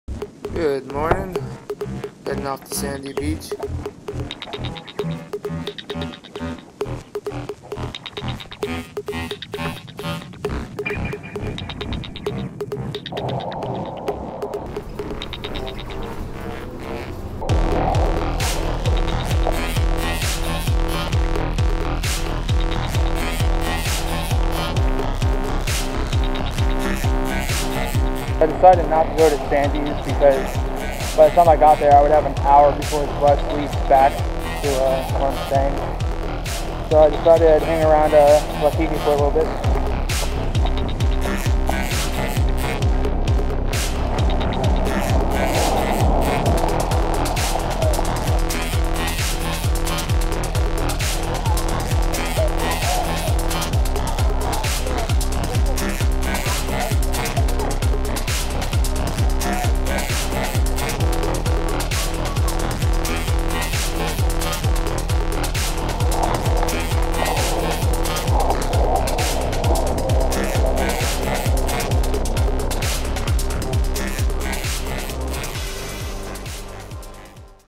First Adventure in Oahu
Spent my first day out in oahu trying to make to sandy beach but ended up spending a little time in waikiki for sundown instead. More adventures/Vlogs to come!